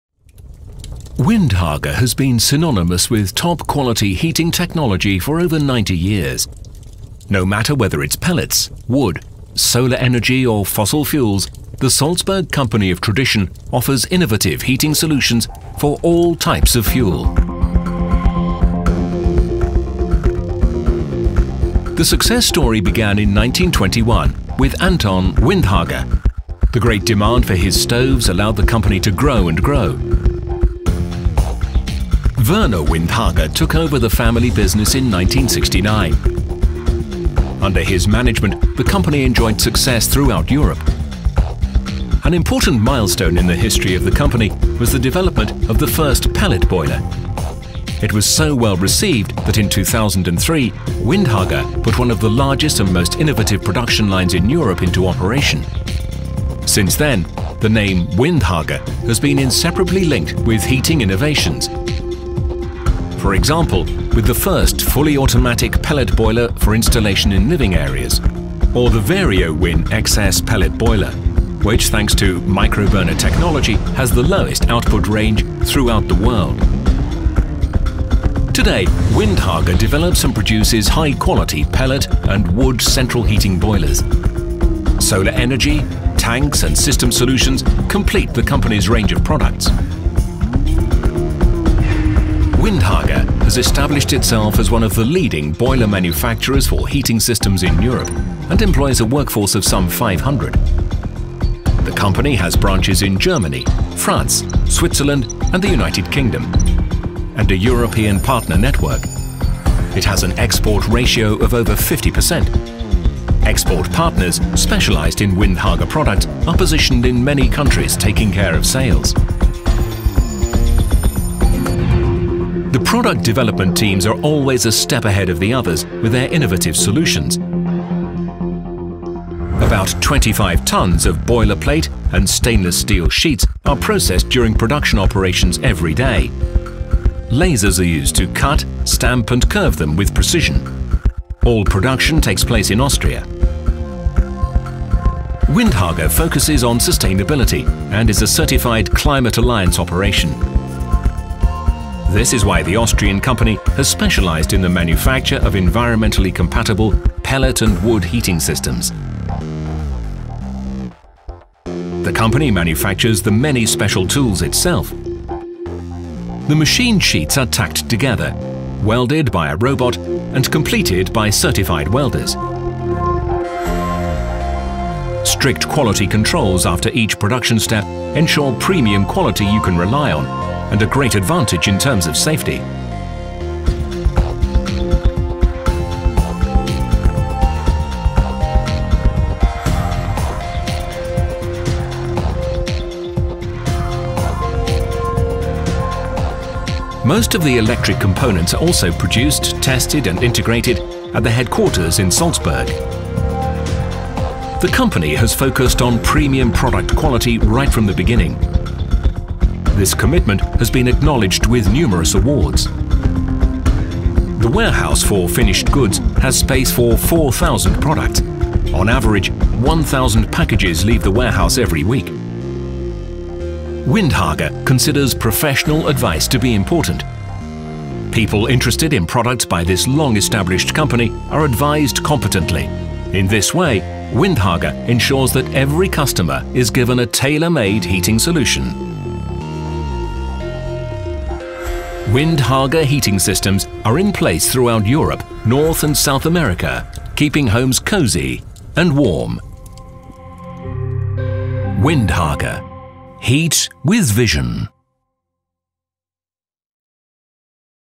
An introduction to Windhager
An introduction movie about Windhager and its production of quality biomass boilers